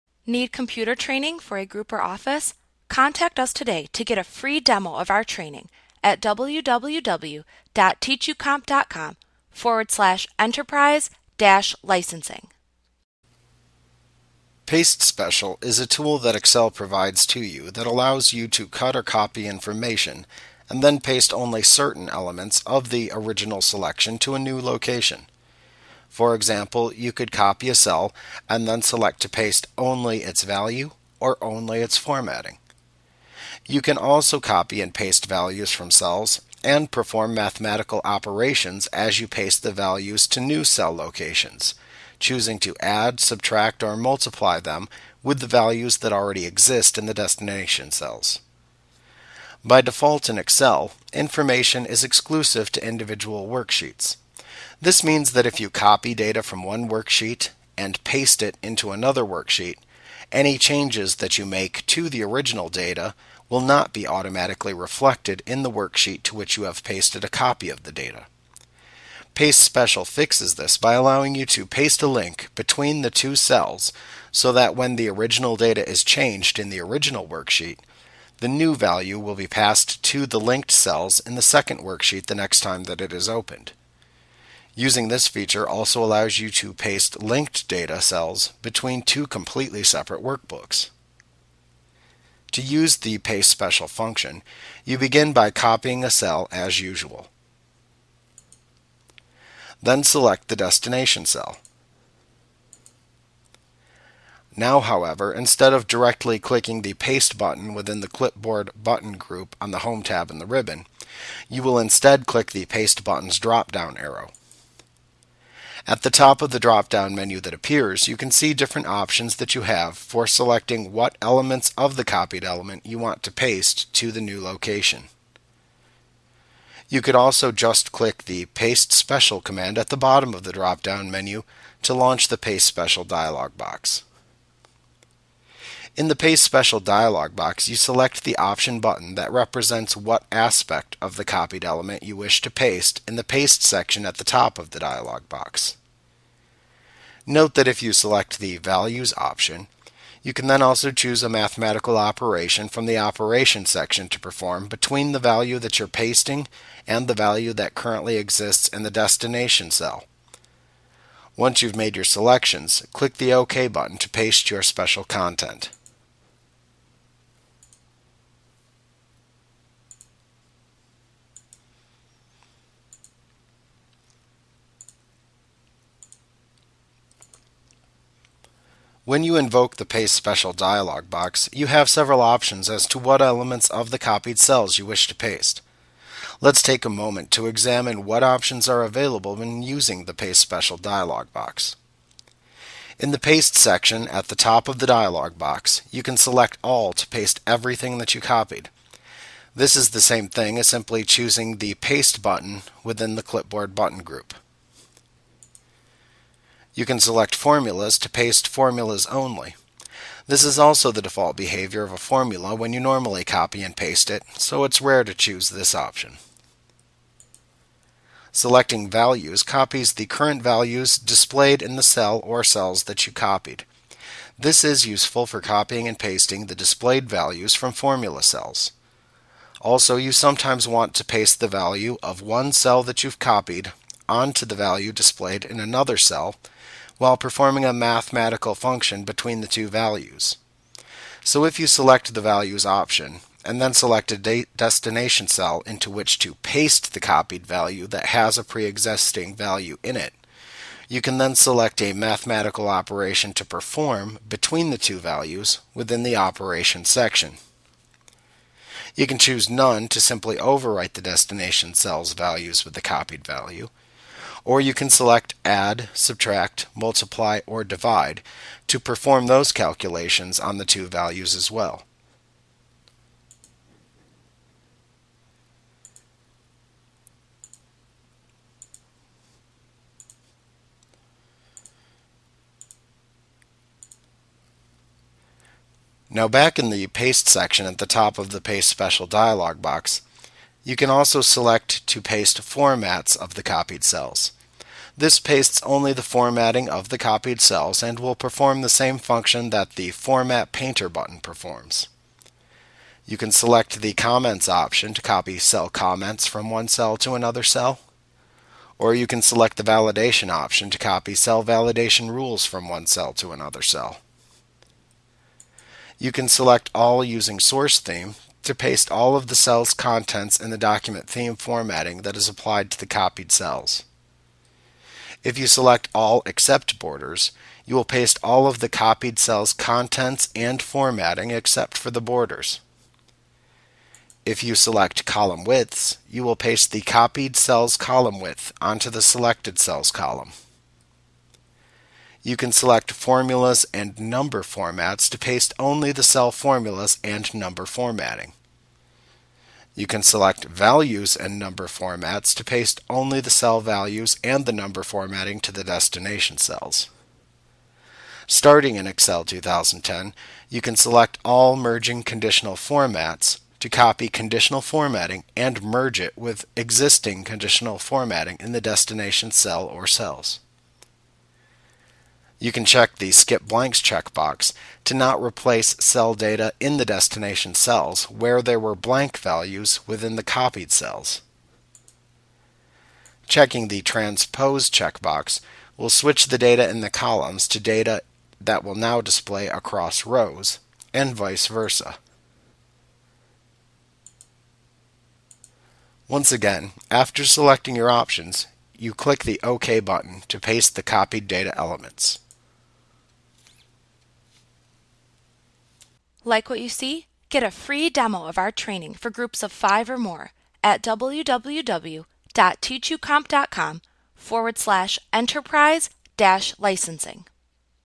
Microsoft Office Excel Tutorial 2013 Paste Special 15.1 Employee Group Training
A clip from Mastering Microsoft Excel Made Easy: how to use paste special. Get a FREE demo of our training for groups of 5 or more